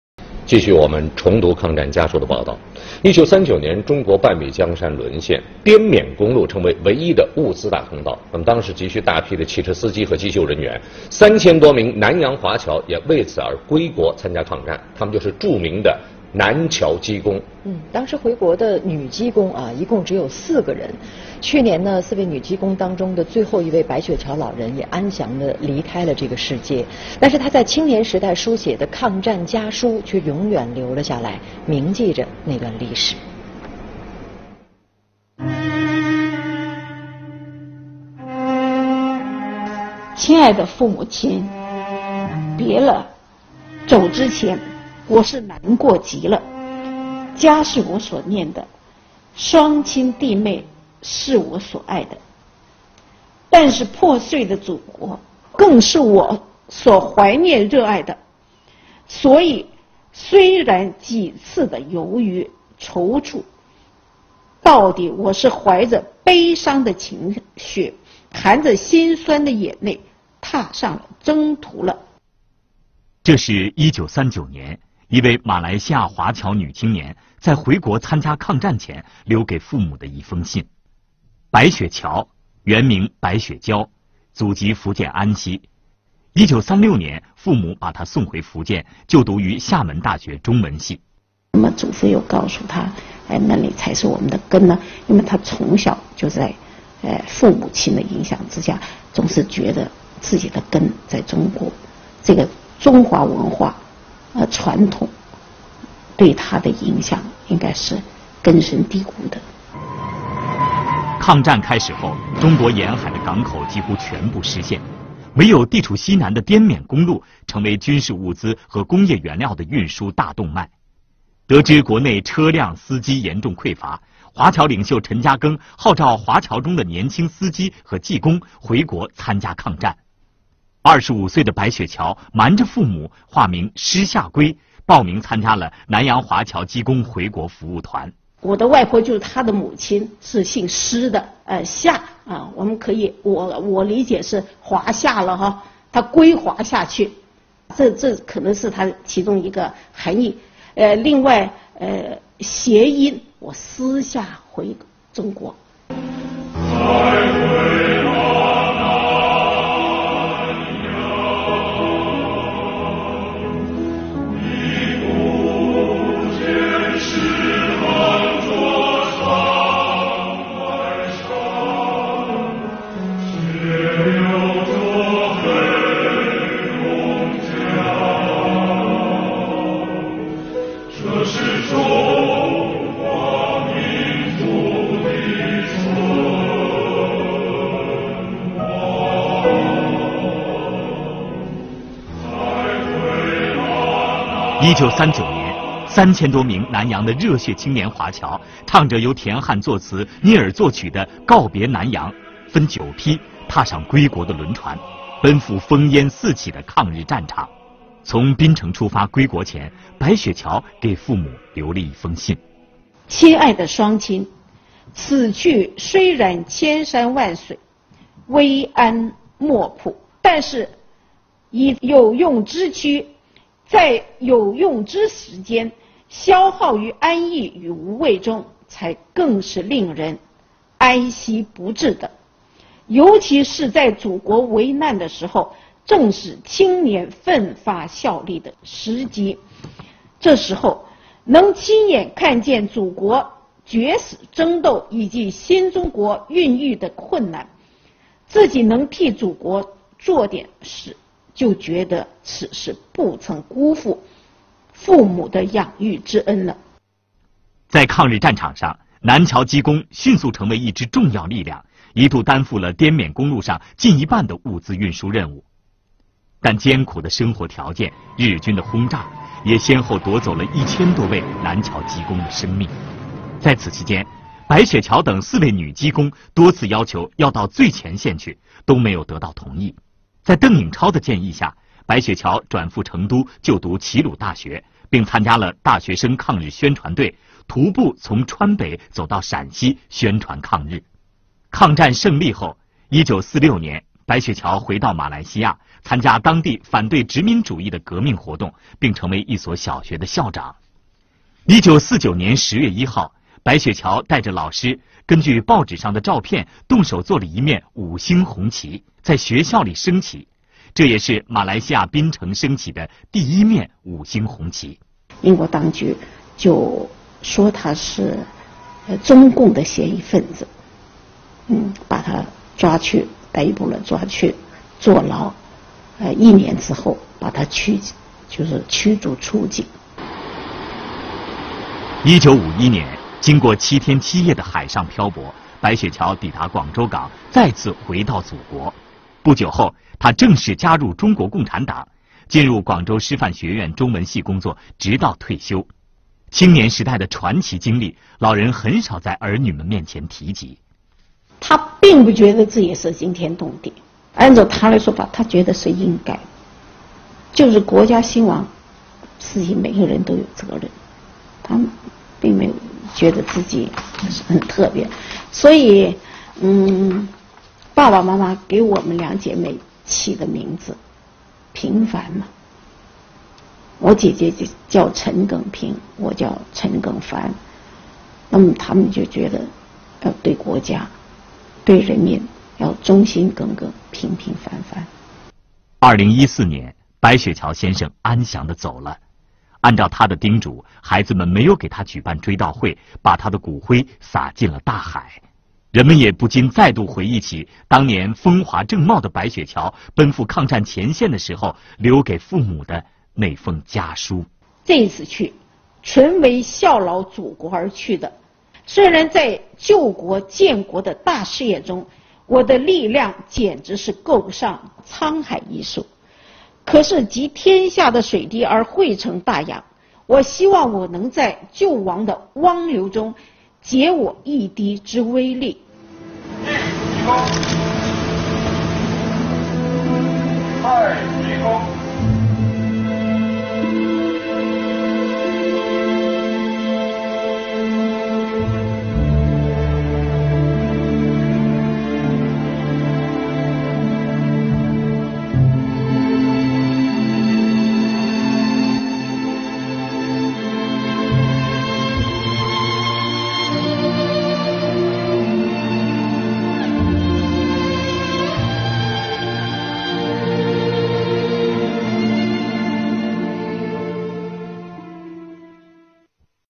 Bai Xue Qiao: Anti Japanese Overseas Chinese Fighter from Malaya
Bai Xue Qiao, who passed away recently in 2014, was one of 4 women from a contingent of 3,200 Overseas Chinese volunteers from Malaya who set sail to China to join the anti-Japanese war in China on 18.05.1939.

These Overseas Chinese volunteers set sail from the Penang port.
At the end of the Chinese Japanese war, 1,000 of them vanished, 1,000 of them stayed back in China and the rest, about 1,200 returned to Malaya.